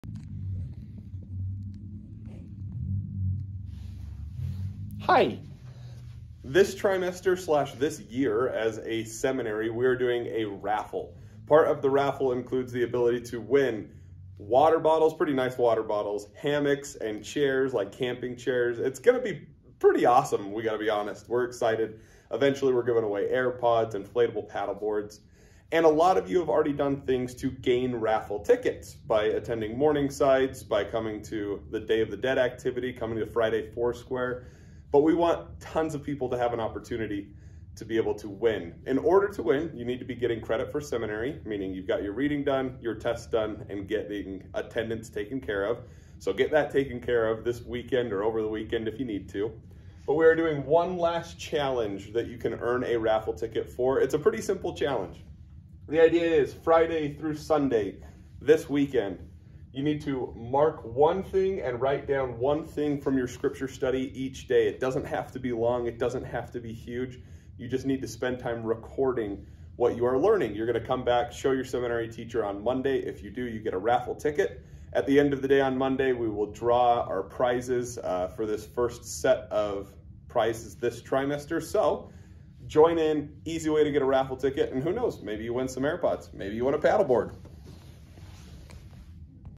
Raffle Reading Challenge Announcement Nov 17 2022
Announcing the Seminary Raffle Reading Challenge over the weekend of Nov 18-20